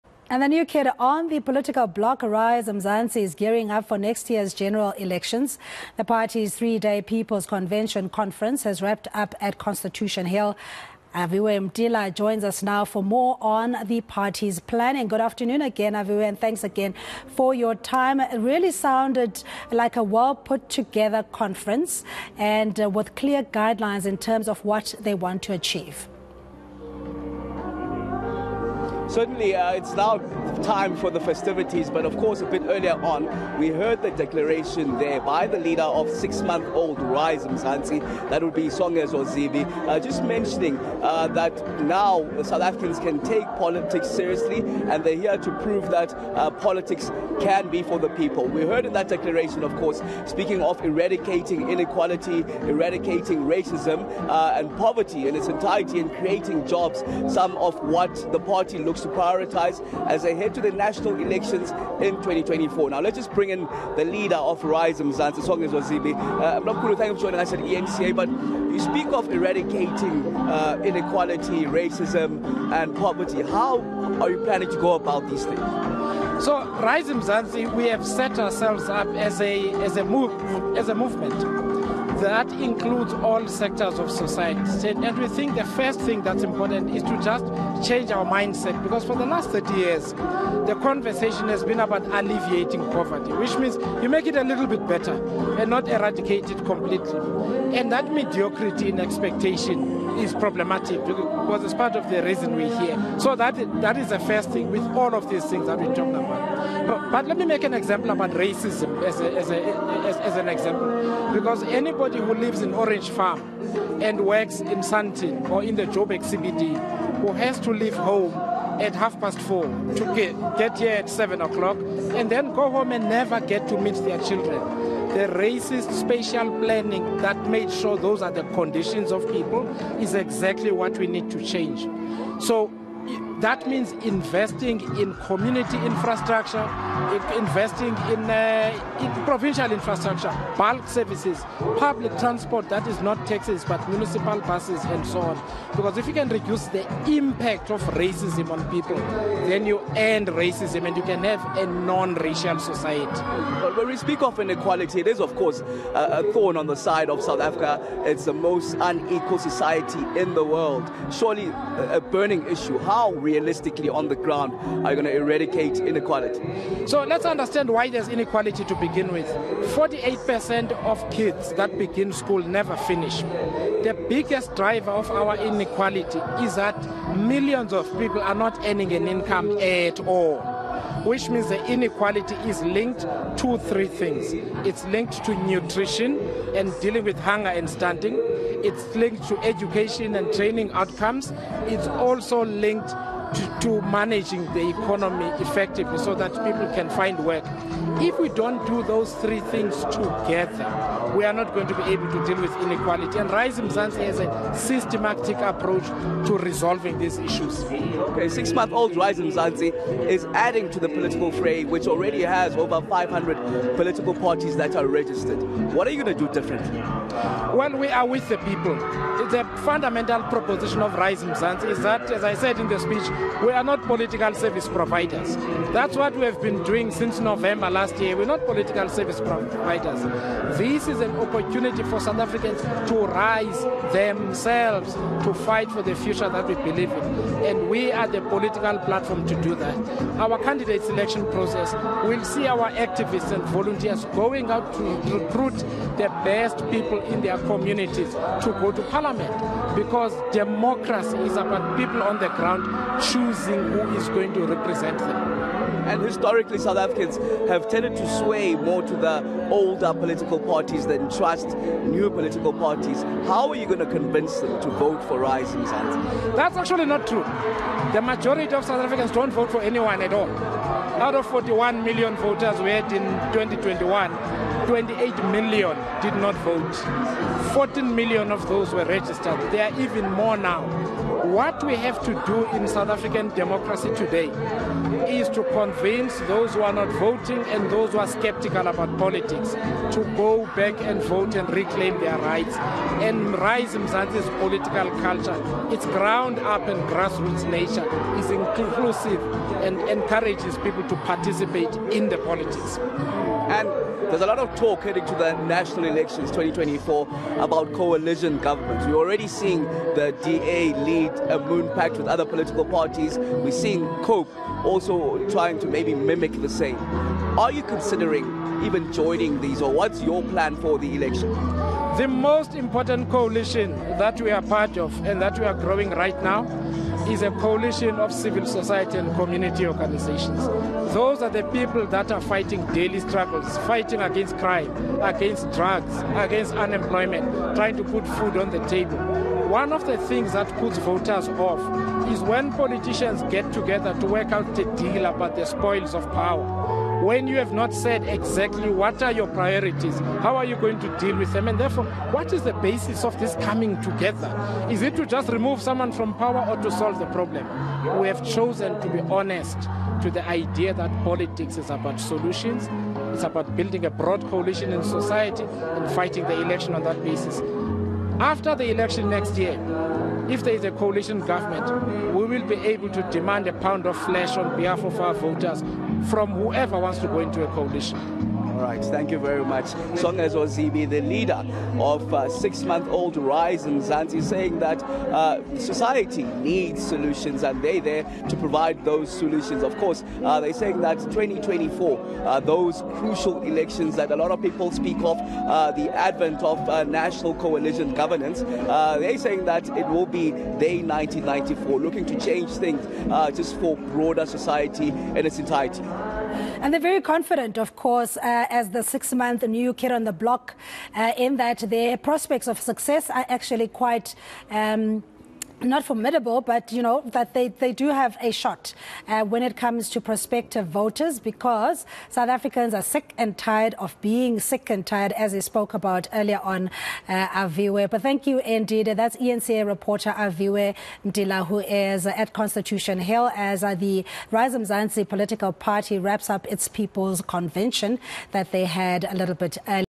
Rise Mzansi | 'The fight won't stop until we get what we deserve' - Zibi
New kid on the block, Rize Mzansi is gearing up for next year's general election. eNCA reporter Aviwe Mtila has the details.